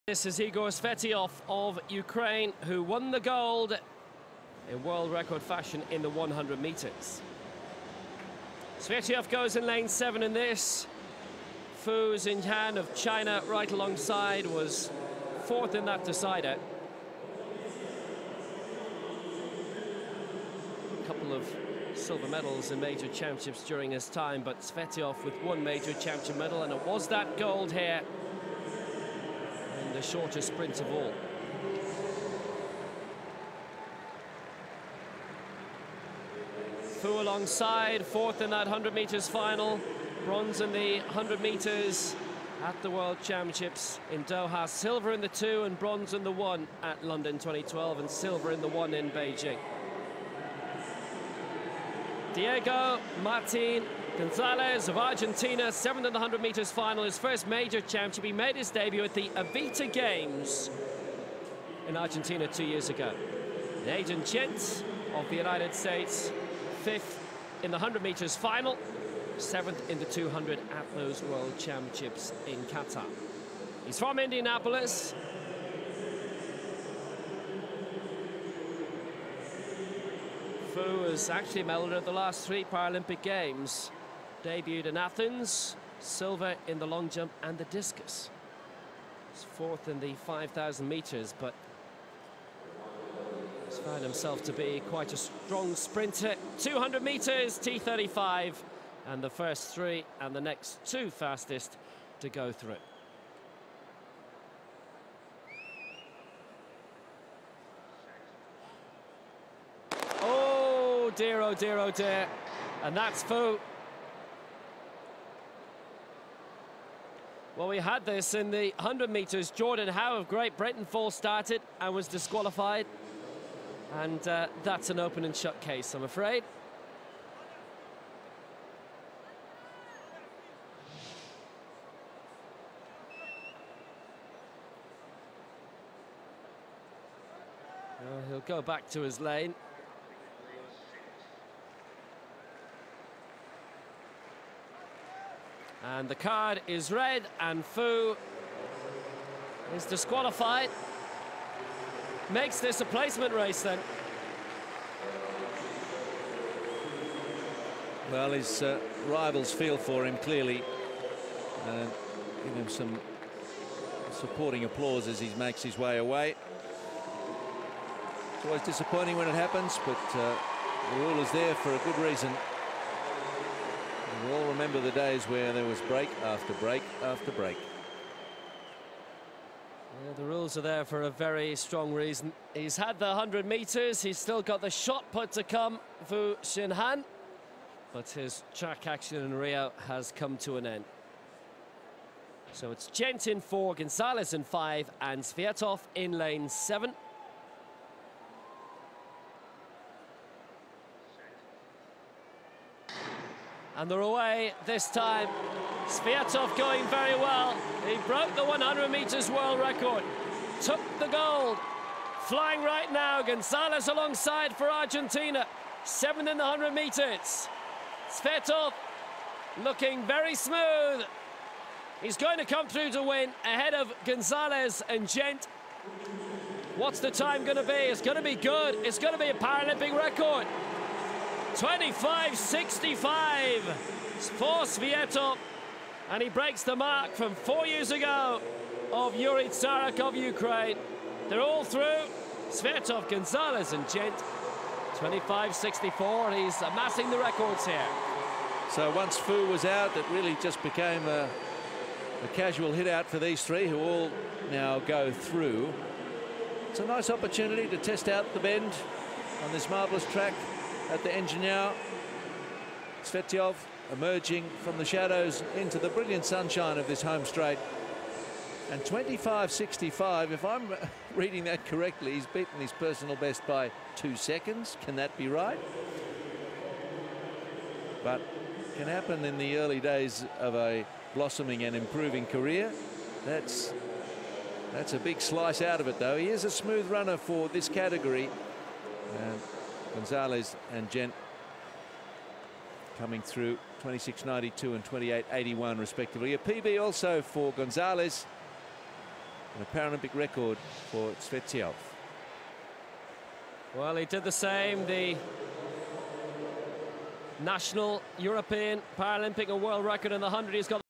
Athletics | Men's 200m - T35 Round 1 Heat 1 | Rio 2016 Paralympic Games
Start lists
4 JENT Ayden (United States of America
5 GONZALEZ Diego Martin (Argentina)
6 FU Xinhan (People's Republic of China)
7 TSVIETOV Ihor (Ukraine)